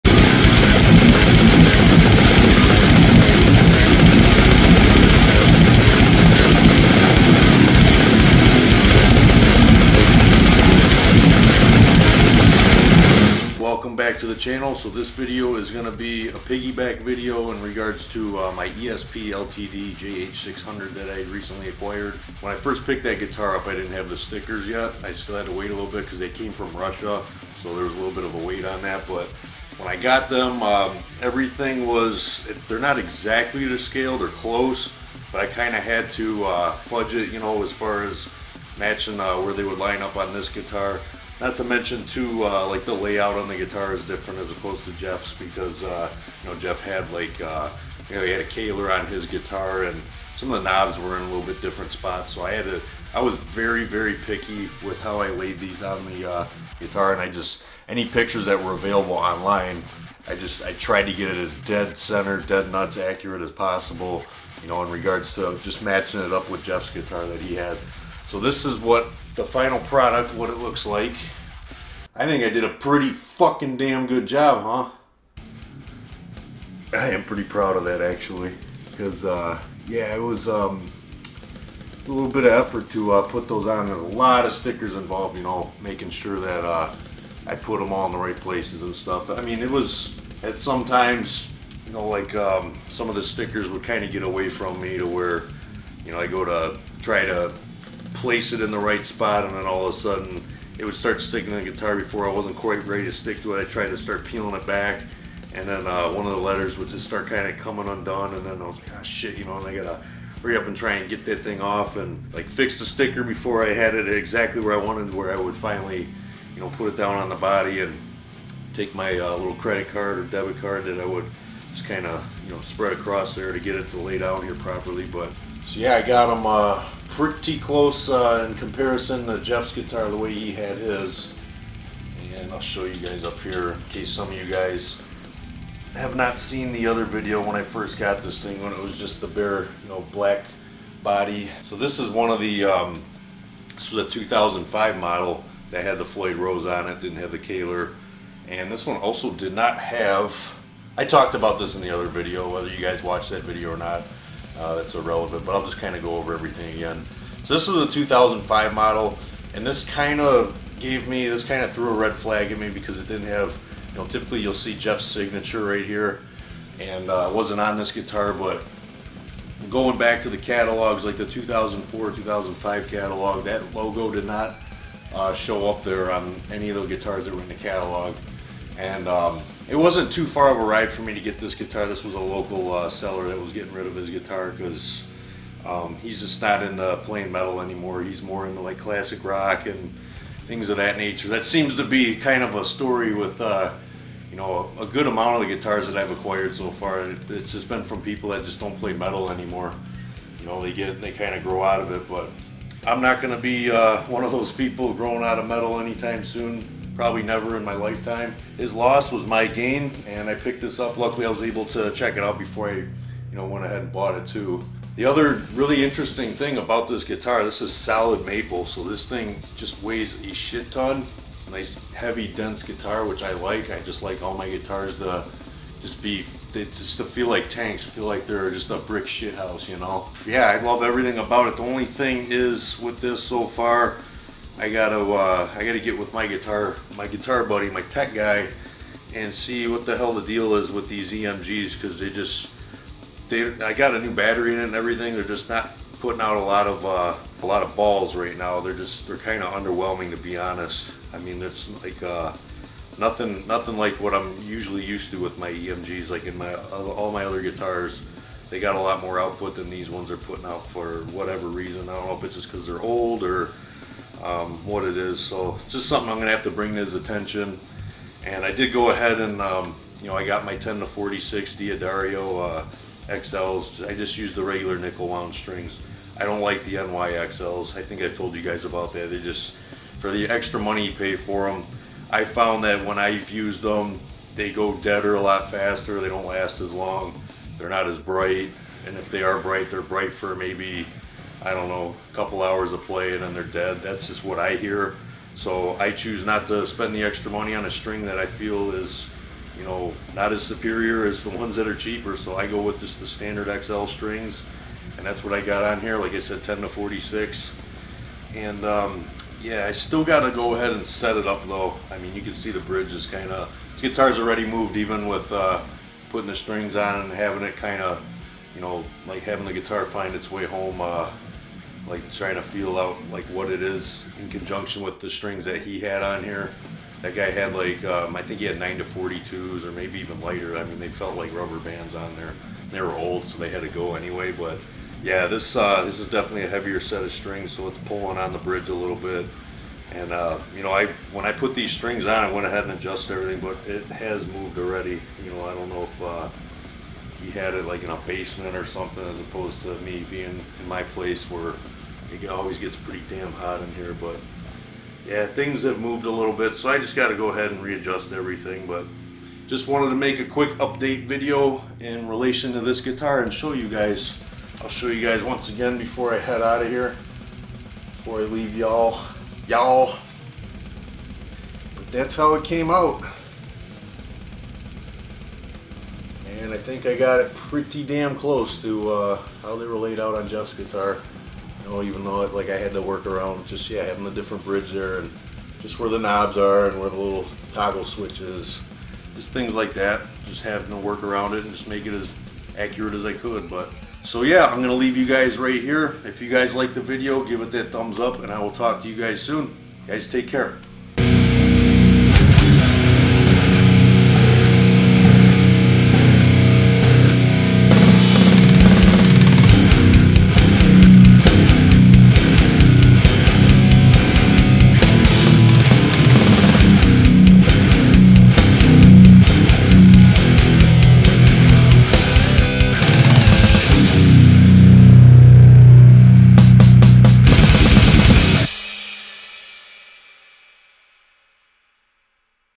Jeff Hanneman Tribute Guitar Completed - CHECK IT OUT!!!!
Here is the completed look of the Jeff Hanneman LTD JH-600 after applying the sticker set.  I'm super happy how it turned out.  Was a lot of fun to do!